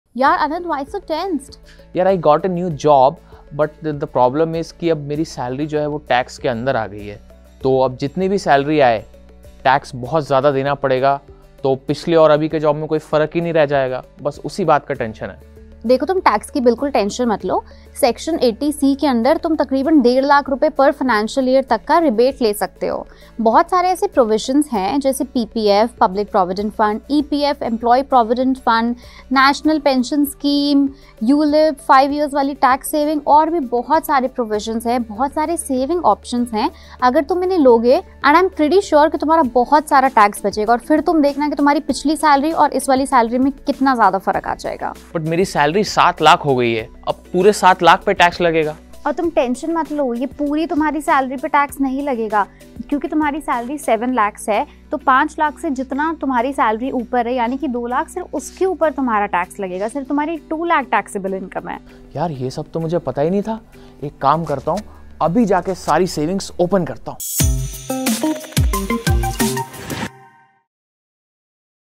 Promotion, Salary Hike से बढ़ा Tax Slab? ऐसे बचेगा ₹ 1,50,000 तक टैक्स | Tips to Save Income Tax
अगर आपकी Salary भी Tax Bracket में आ गयी है तोह Section 80C आपकी taxable income Rs 1,50,000 तक कम कर सकता है. देखिये कौनसे deductions और exemptions offer  करता है section 80C